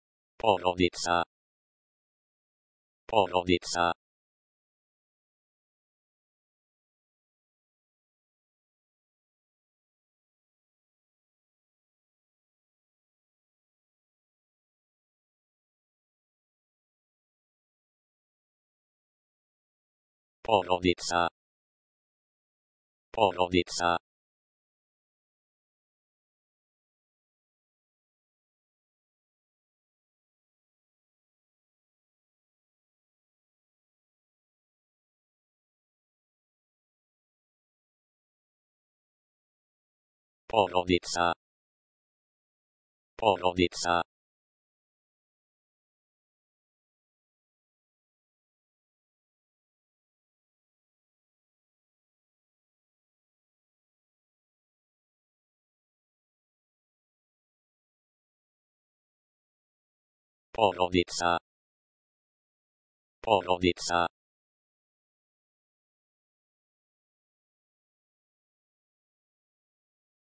Wie sagt man Familie auf Bosnisch?
Wie sagt man Familie   auf Bosnisch? Lerne die Aussprache von Familie  !